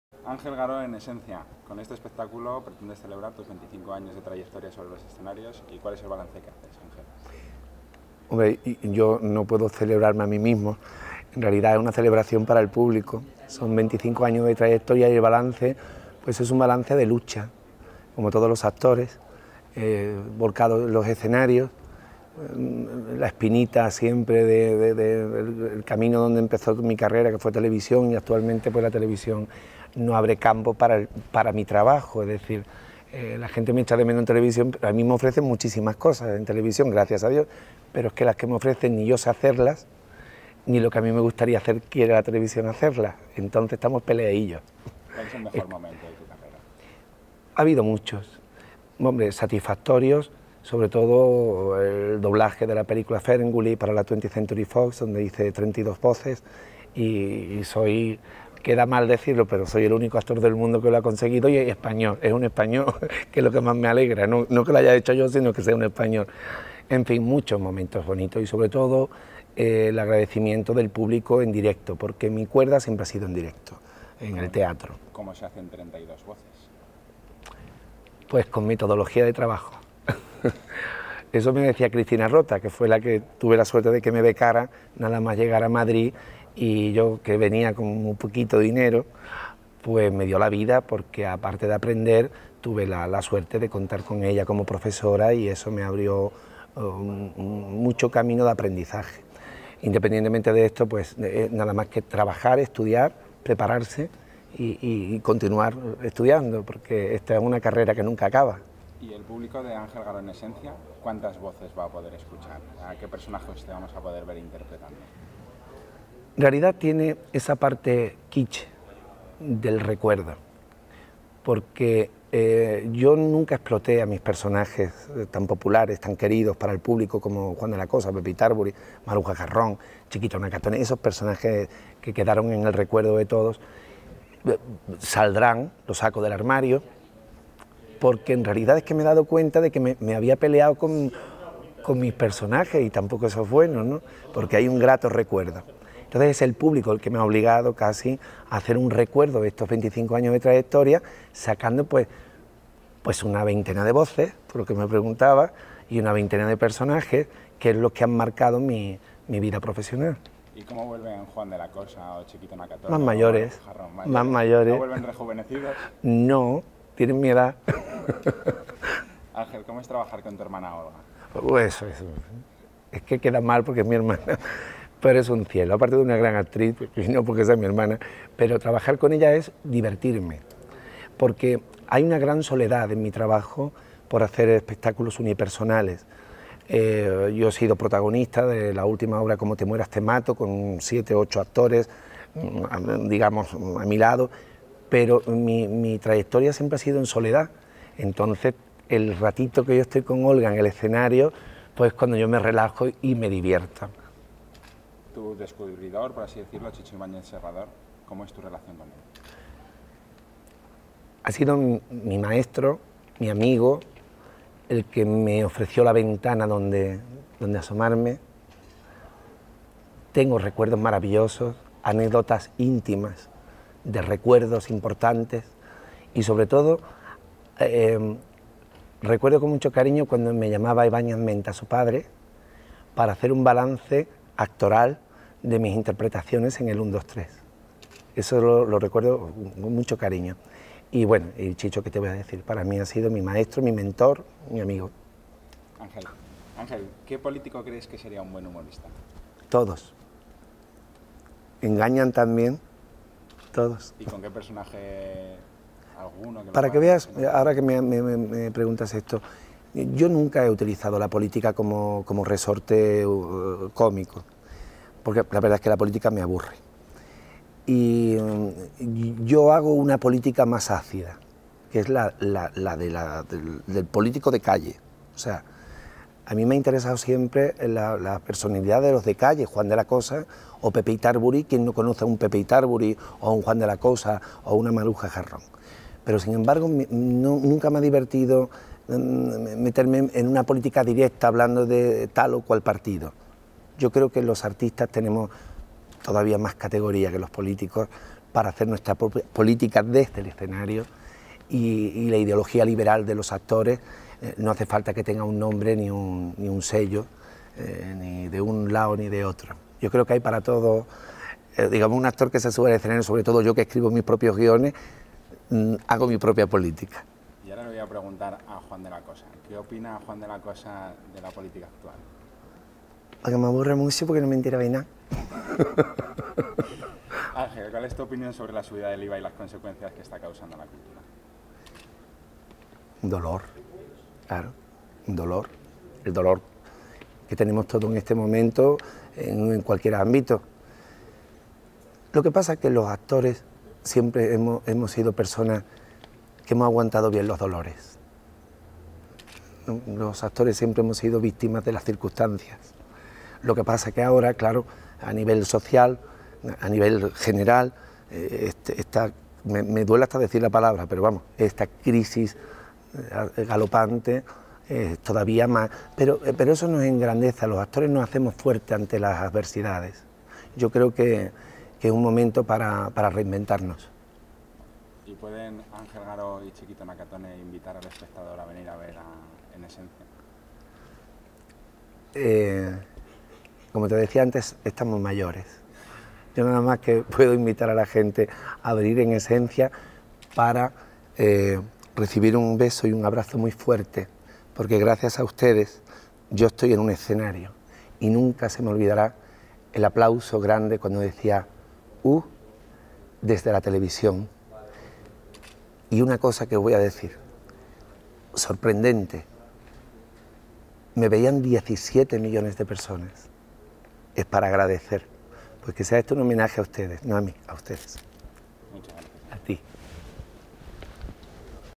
Entrevista Ángel Garó - "En esencia"
"Ángel Garó. En Esencia" es una retrospectiva de la actividad profesional realizada durante los años que Ángel Garó ha permanecido en los escenarios y el mundo de la televisión. Un recorrido de dos horas de duración que efectúa con todos los personajes de su creación que han hecho historia dentro del mundo de teatro cómico y de la televisión. Acompañado por la actriz Olga Garó, te hará disfrutar de un espectáculo divertido y apto para todos los públicos. Maruja Jarrón, Marutxi, Juan de la Cosa, Chikito Nakatone y la fuerte personalidad escénica de Ángel Garó hacen de este espectáculo algo especial.